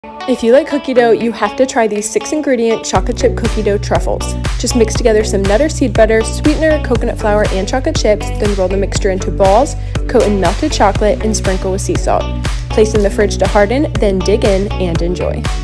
6 ingredient chocolate chip cookie dough truffles
chocolate chip cookie dough truffles! they’re gluten free, dairy free, and refined sugar free, and so easy to make!

ingredients:
•nut or seed butter of choice (i used almond butter)
•pure maple syrup or raw honey
•pure vanilla extract
•salt
•coconut flour
•chocolate chips (i use dairy free and refined sugar free)

happy weekend!